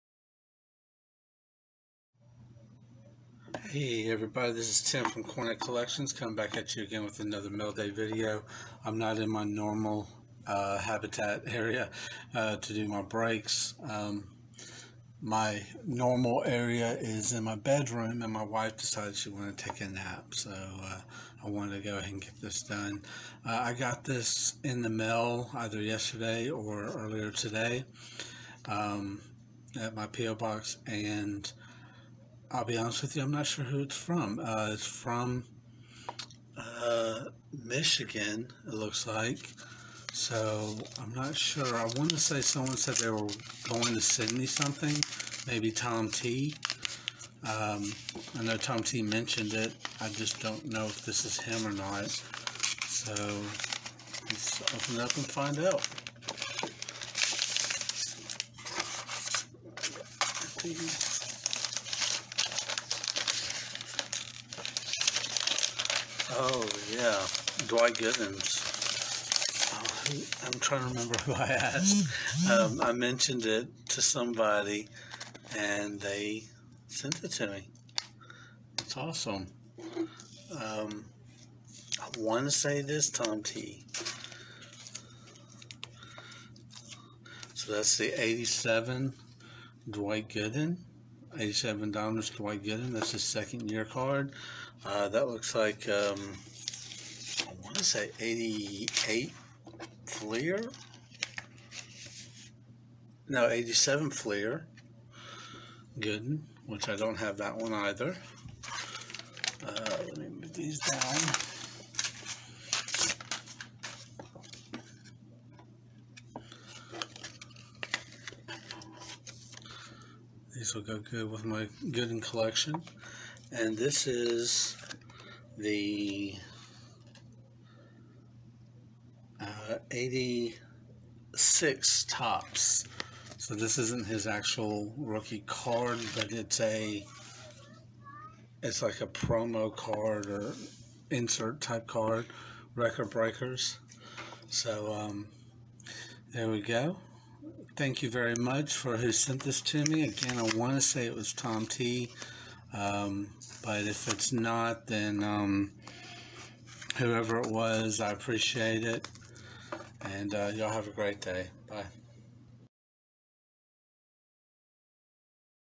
Mail Day: Mystery Mail (possibly from Thom T)
I received a piece of mail from whom I believe is ThomT. I watched a video and he left a comment about a few of the cards. I told him I PC Dwight Gooden and he sent them to me. Check out his videos where he combines cards with comic books.